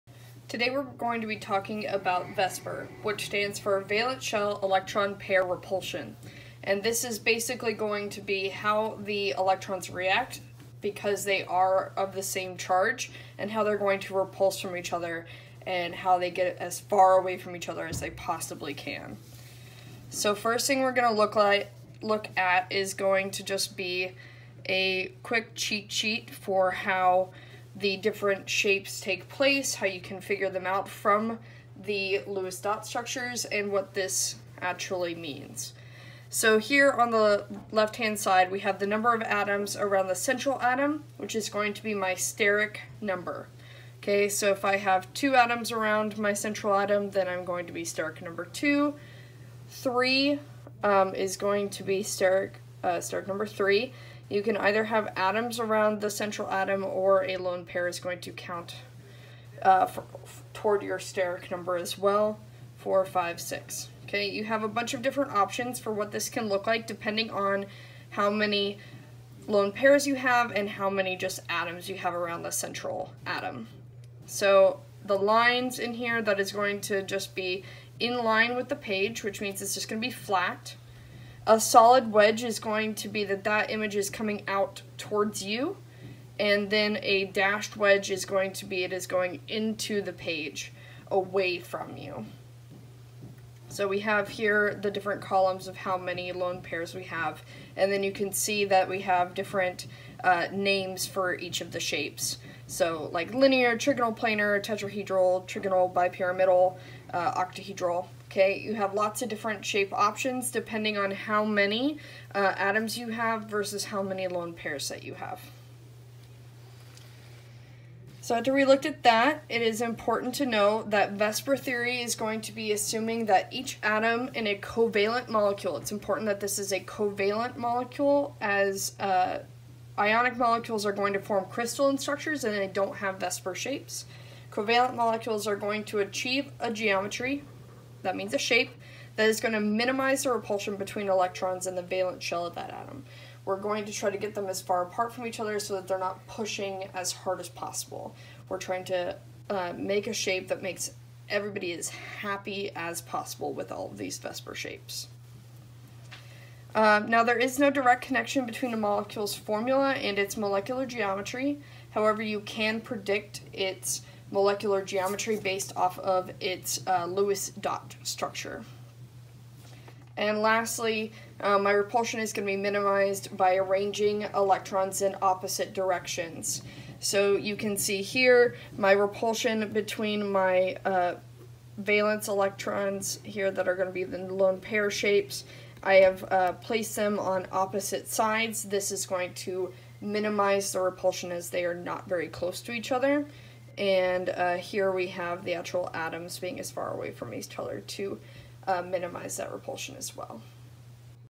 VSEPR
Basic VSEPR structures and explanation as to why VSEPR exists akins high school chemistry